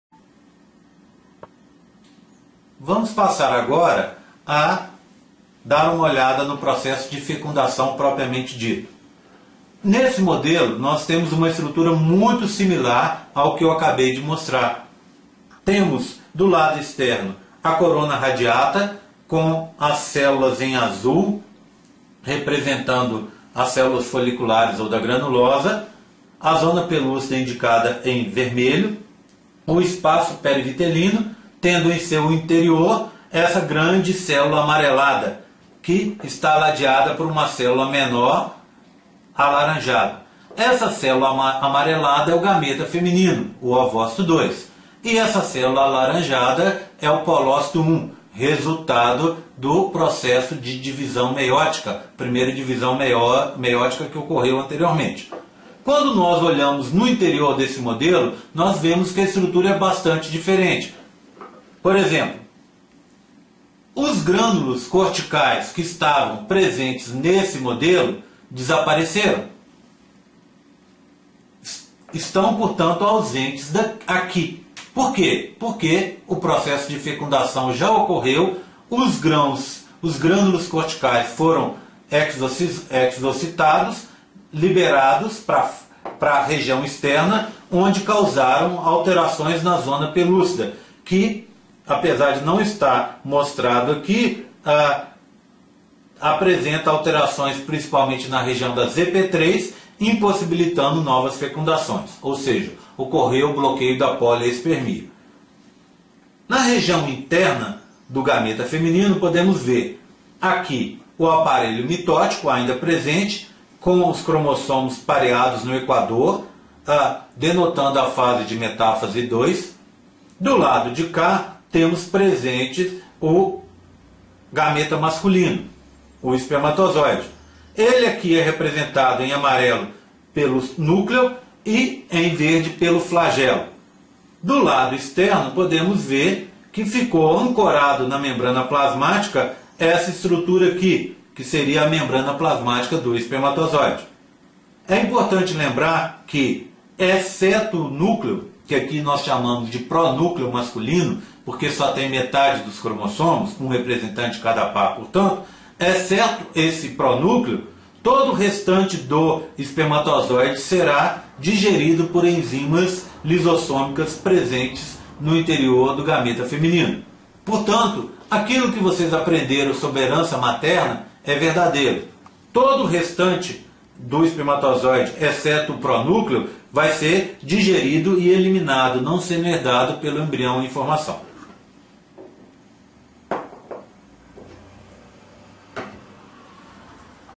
fecundação.mp4
Vídeo descrevendo modelo didático tridimensional sobre a fecundação humana.